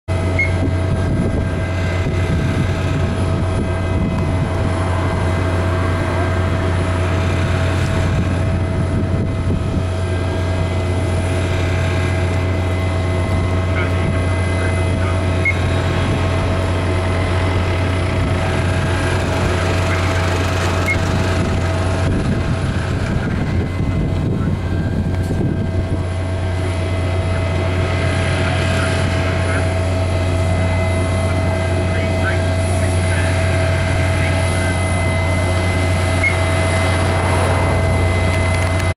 Wadhurst, East Sussex Fire & Rescue Service
Wadhursts Maxi Cab ERP turning up at a MP 8, L4T 5, WrC 1 forest fire on the Ashdown Forest